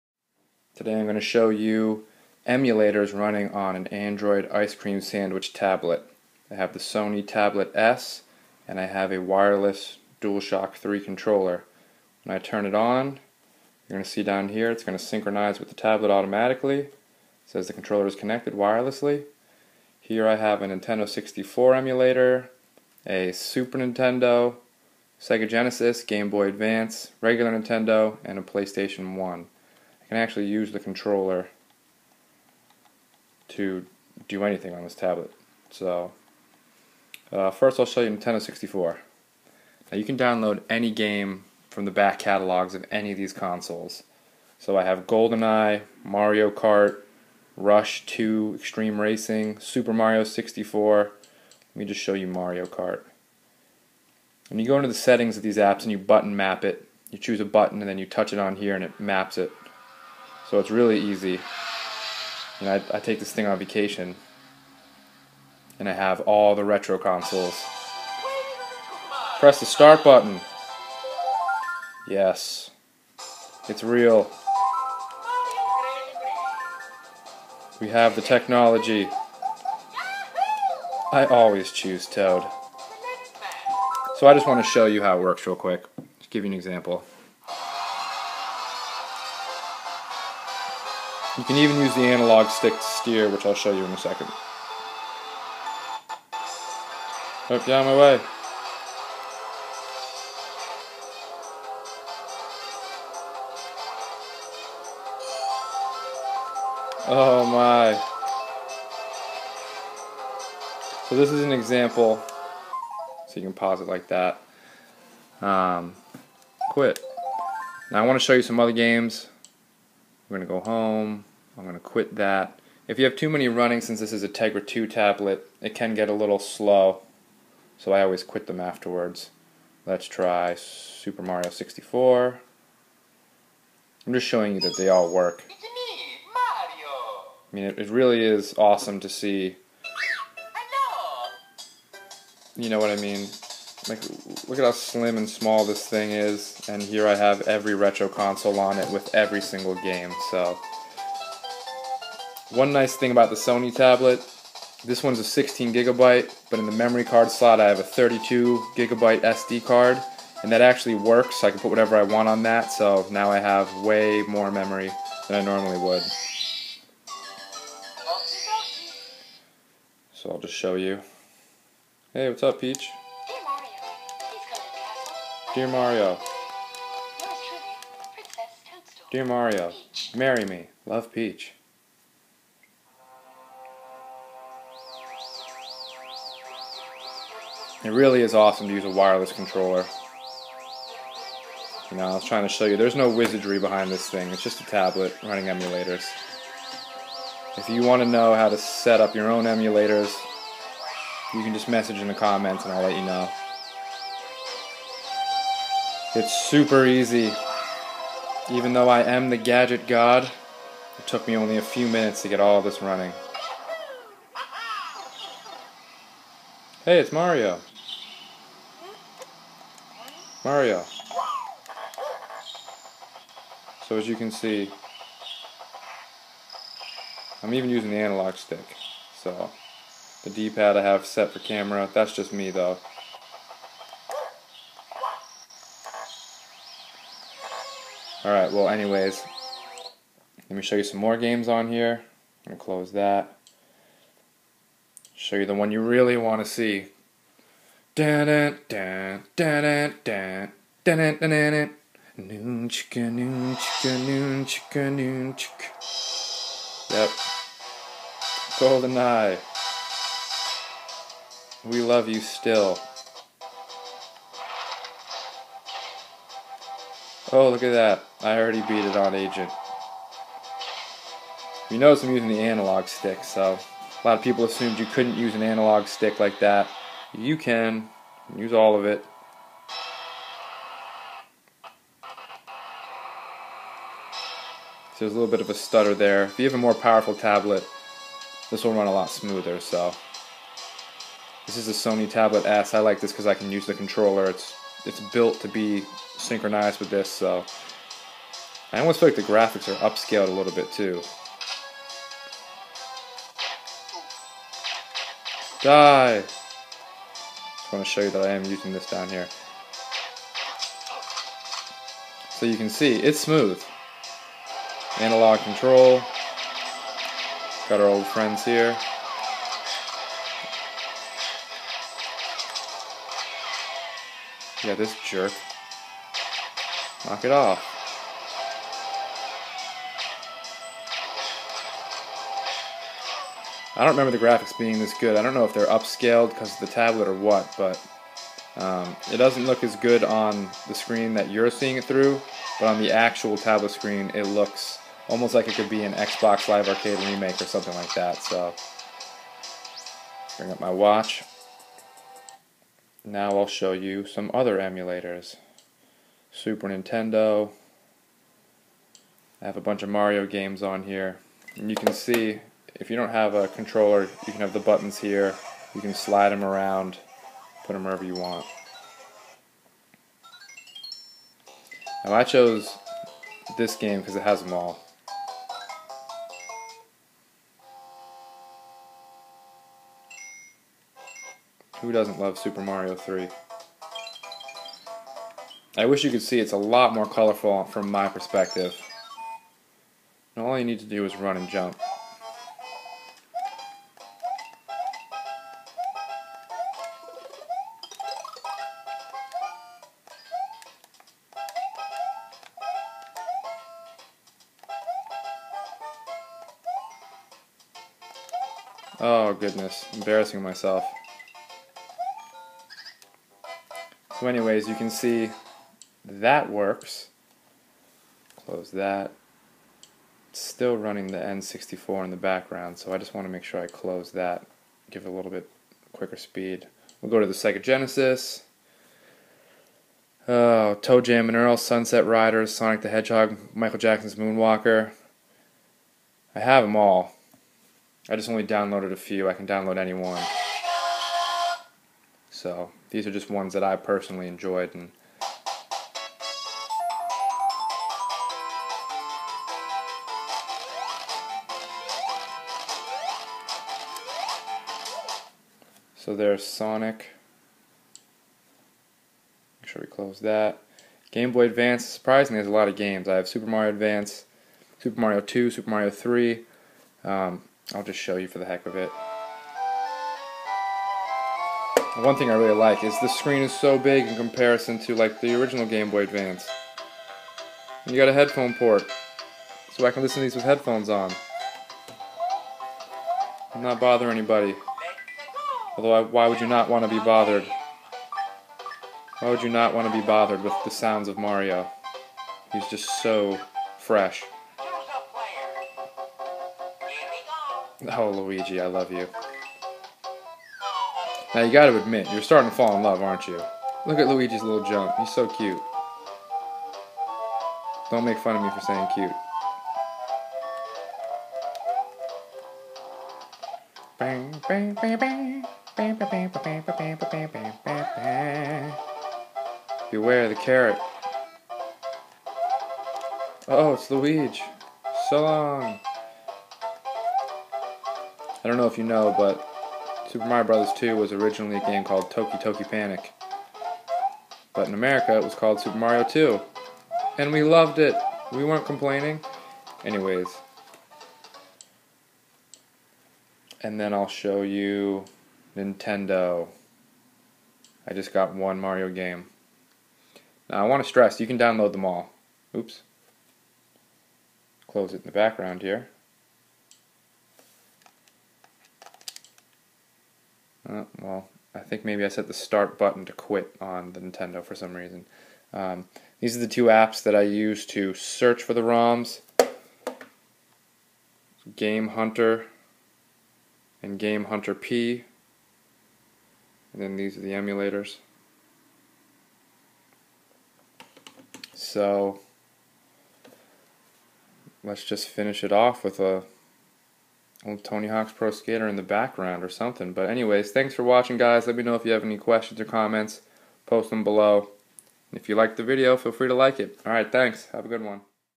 NINTENDO EMULATOR ON ANDROID w/ PS3 Controller Wirelessly
Android emulators on a Sony Tablet S running Google's Ice Cream Sandwich software using a wireless DualShock 3 Playstation 3 controller.
This is the Sony Tablet S, which came out before the current Sony Xperia Tablet. Both Sony tablets are designed to be able to seamlessly and simply sync with a Playstation 3 Dualshock 3 controller. You need any brand USB cable in order to connect the Playstation 3 controller to the Sony tablet the first time. After you have sync the two devices together, you only need to turn on the Dualshock 3 controller to have it connect wirelessly automatically.

Gadget Deals